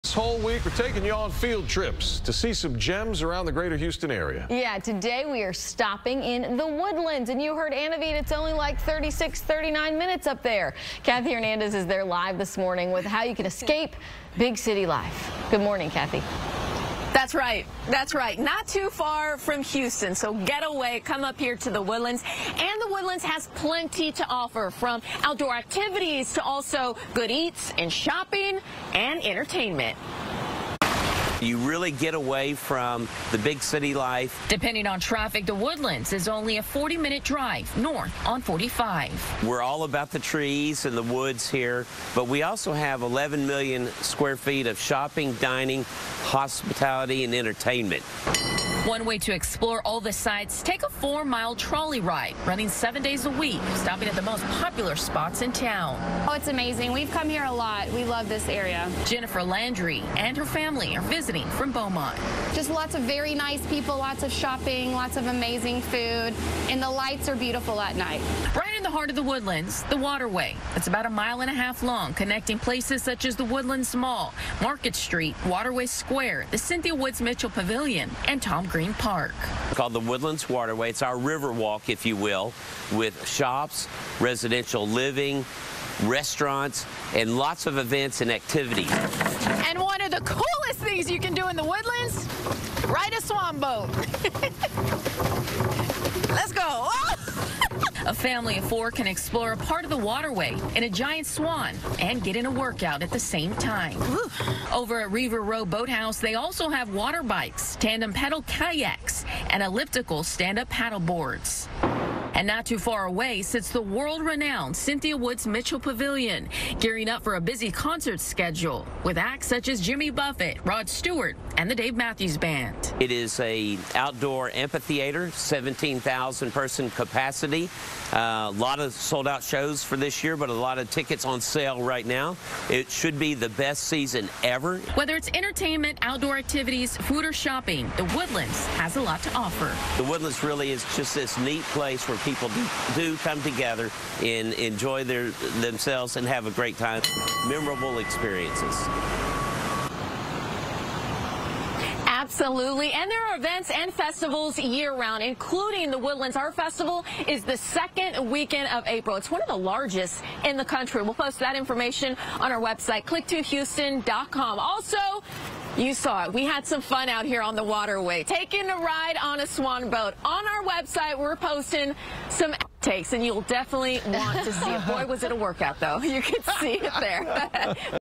Field Trip February: Escape big city life in The Woodlands
Get away from big city life to experience gems in The Woodlands.